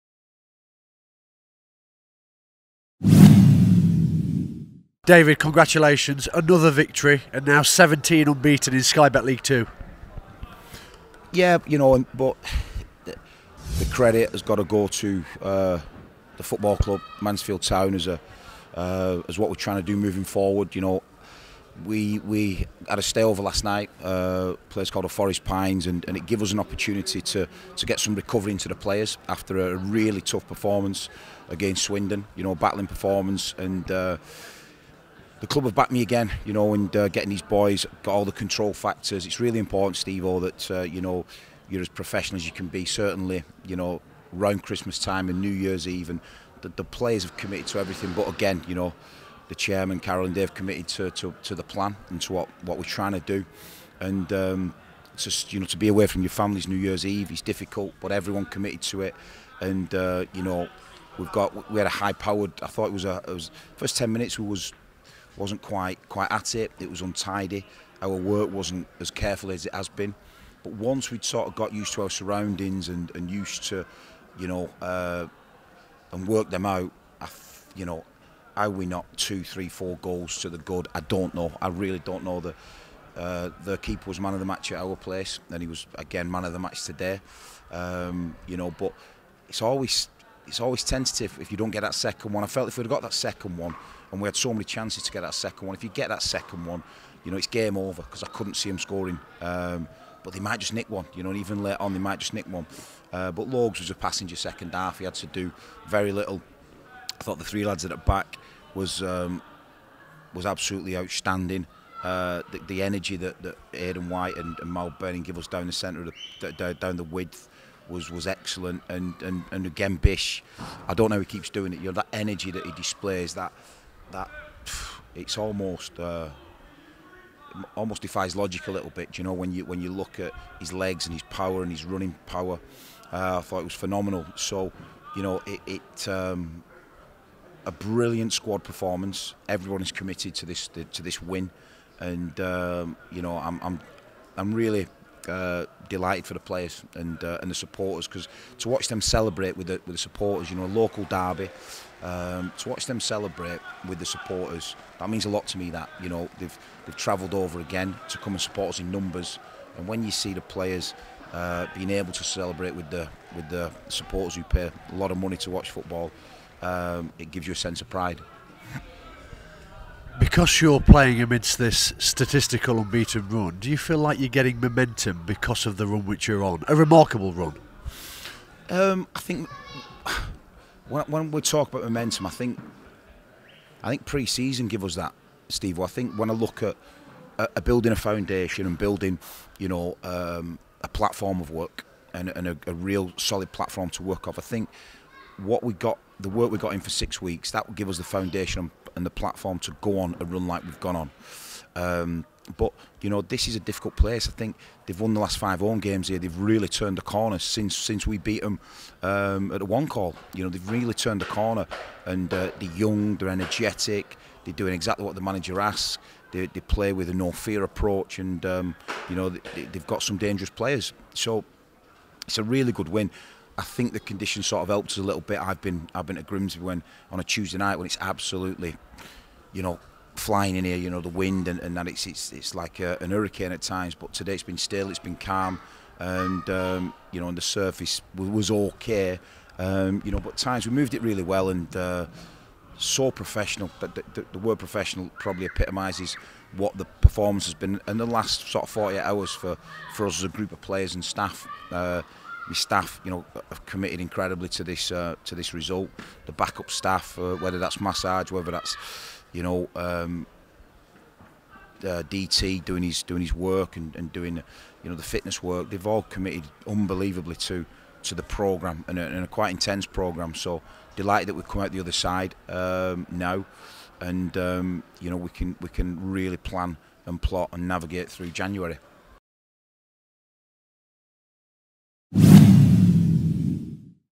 Stags’ gaffer hails squad after win at Grimsby
FREEVIEW: Stags’ boss David Flitcroft hails an ‘incredibly professional, squad performance’ after a 1-0 victory at Grimsby sees us extend our unbeaten run to 17 games in Sky Bet League Two.